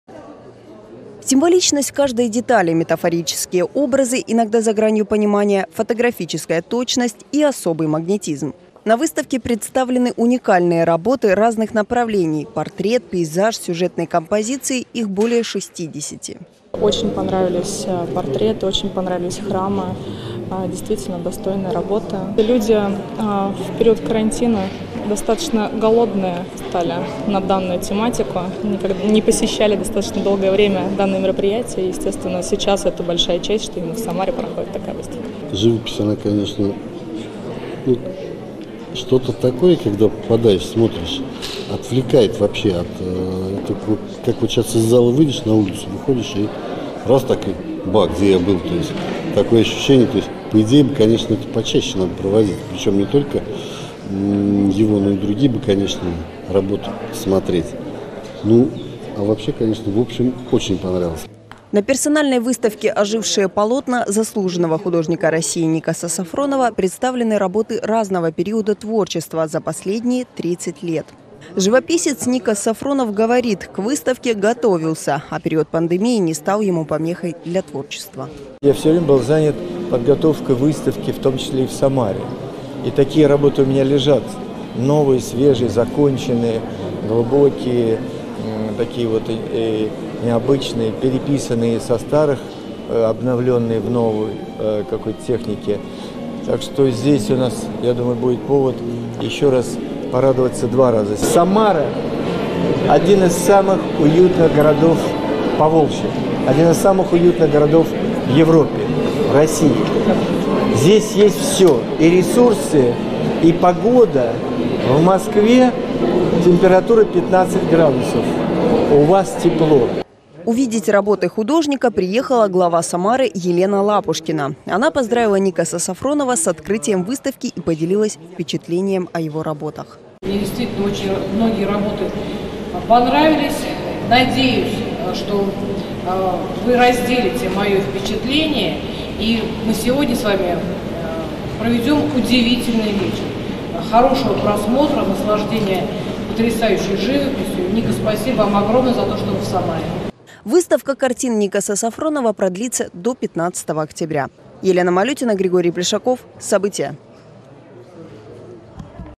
Открылась выставка Никаса Сафронова
Окунуться в уникальный мир известного художника с мировым именем. В музее Алабина открылась выставка Никаса Сафронова. Гости и жители города смогут увидеть более 60 оригинальных полотен мастера. Подробности в сюжете Елены Малютиной.